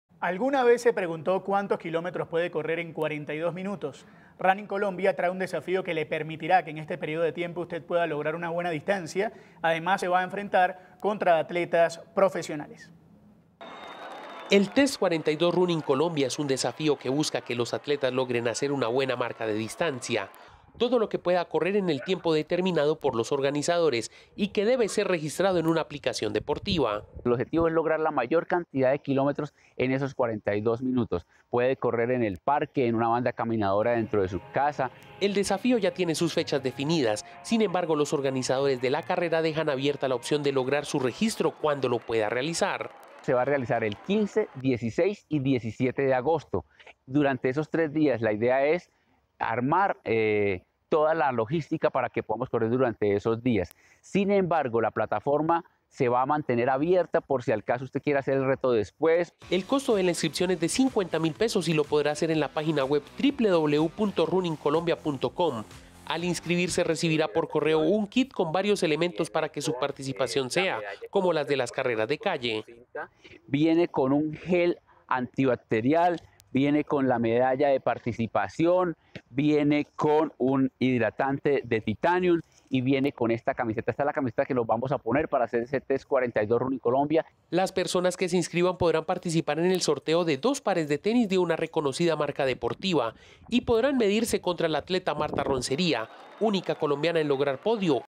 Test 42, una carrera virtual
DeportesCapital | Las inscripciones estás abiertas para esta carrera virtual que consiste en correr la mayor cantidad de kilómetros en 42 minutos.